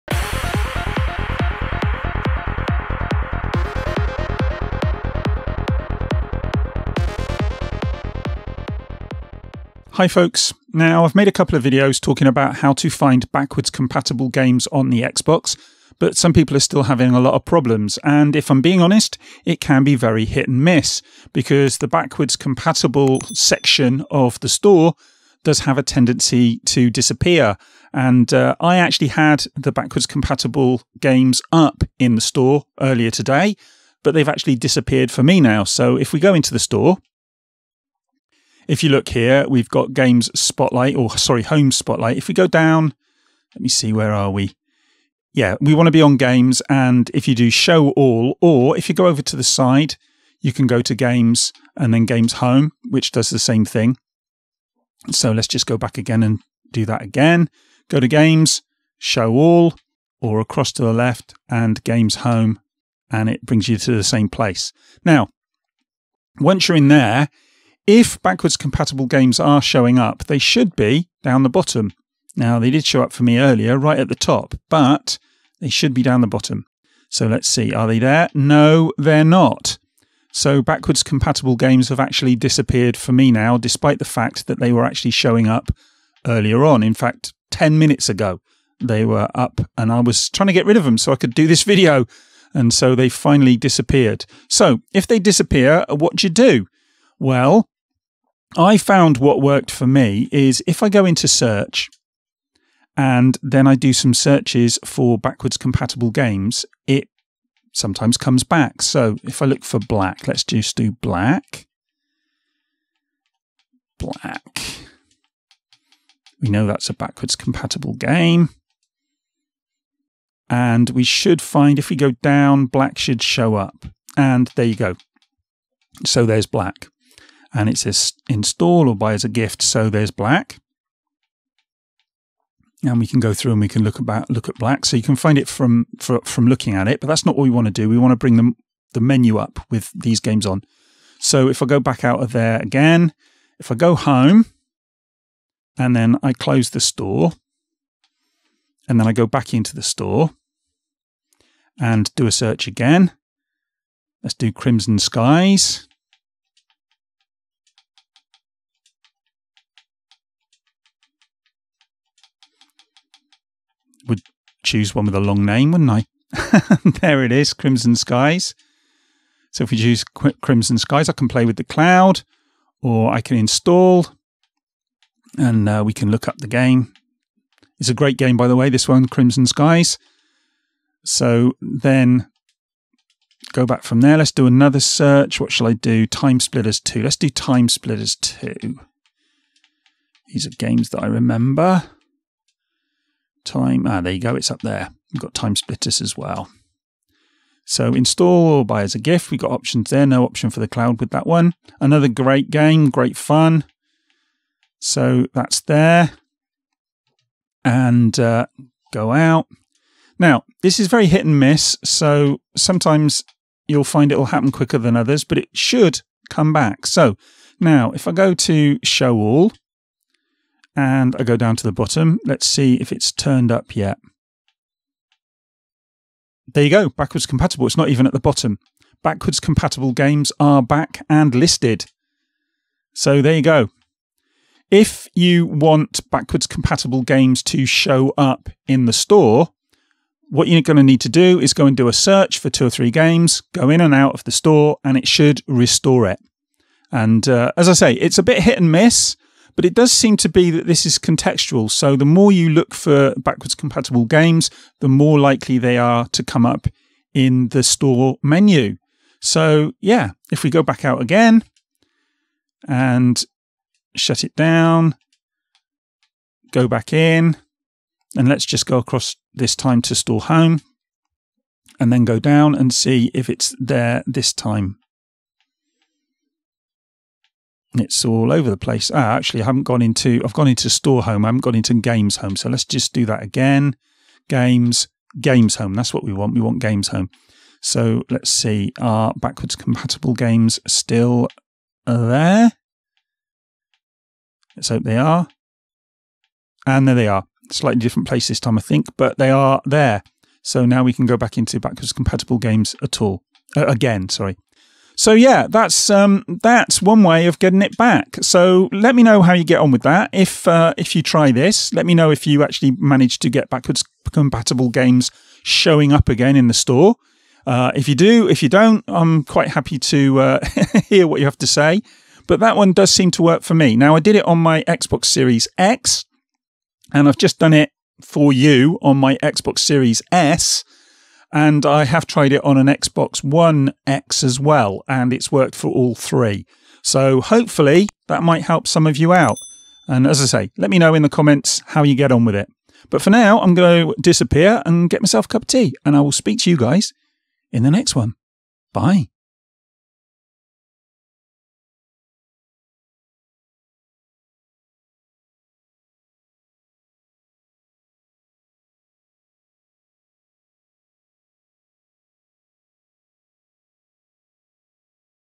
Force Backwards Compatible Games to Appear in the Xbox Store
Backwards Compatible games keep disappearing from the Xbox Store, making it hard for people to purchase them. Here's a way to get them to show up again.